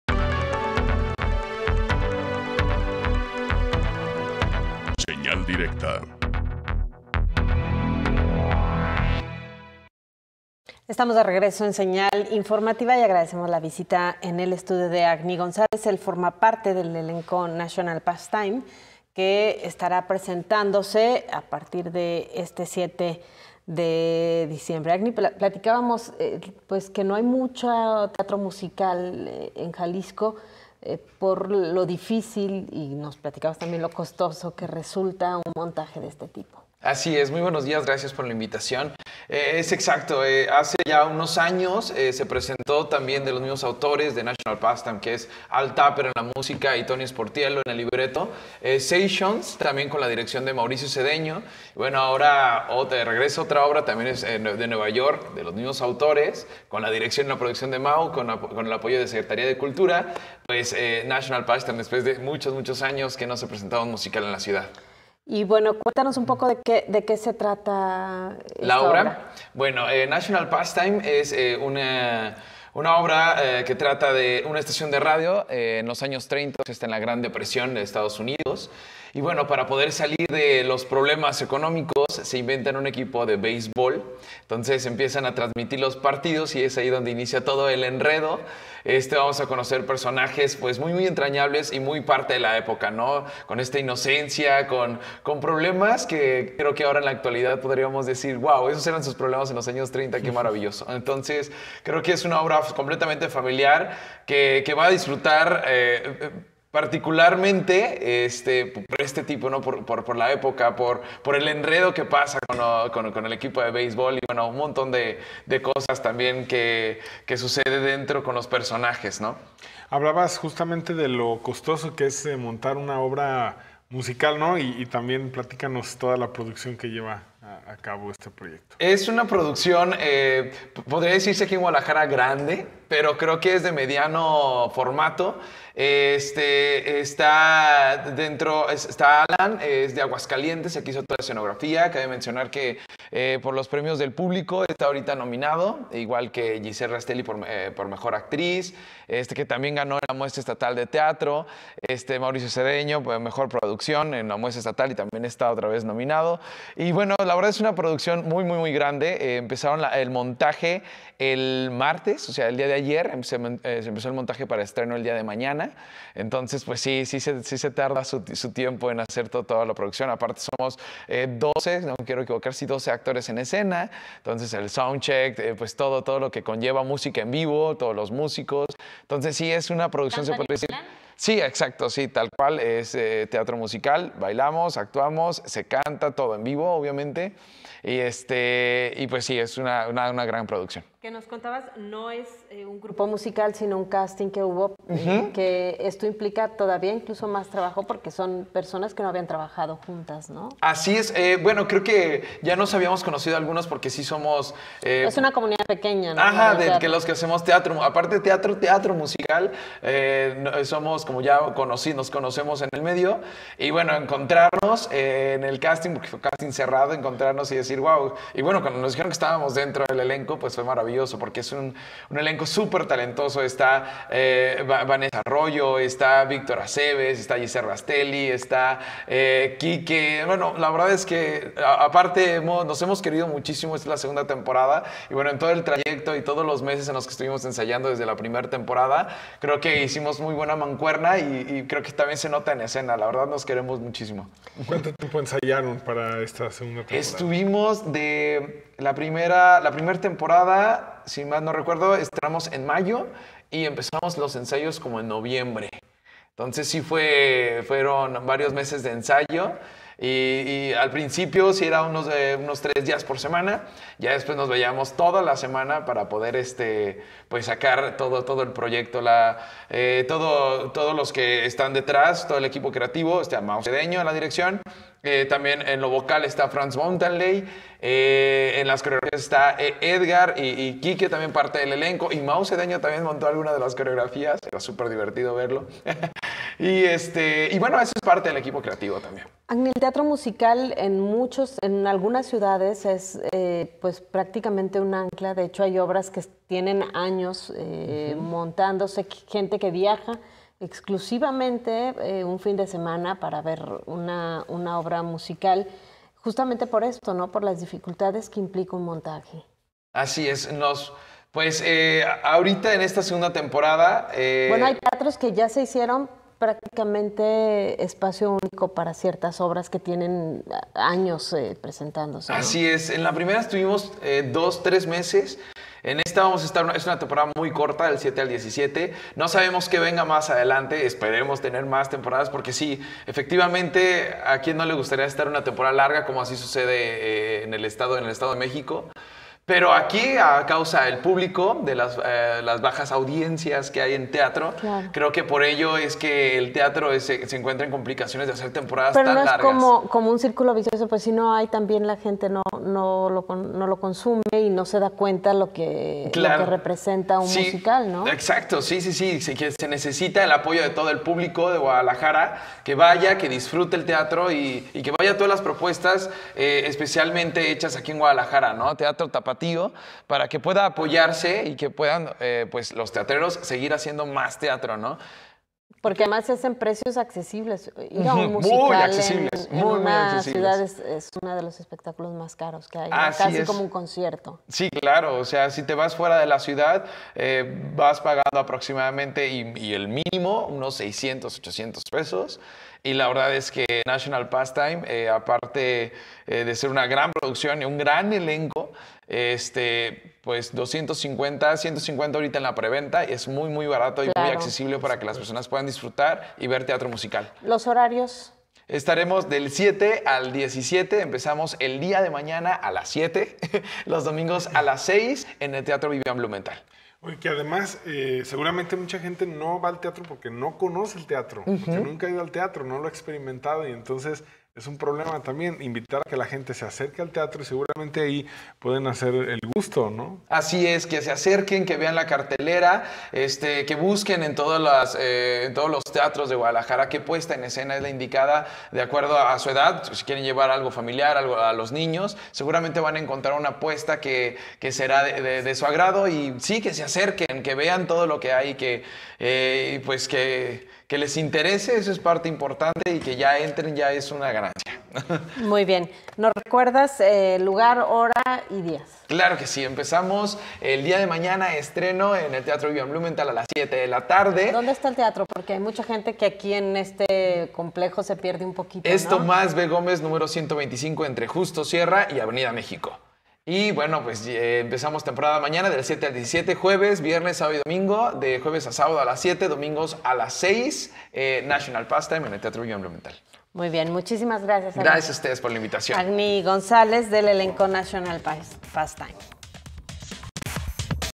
Agni González "Invitación al público a la obra National Pastime"
Está en nuestro estudio Agni González,  quién forma parte del Elenco de National Pastime que se presenta próximamente en Guadalajara: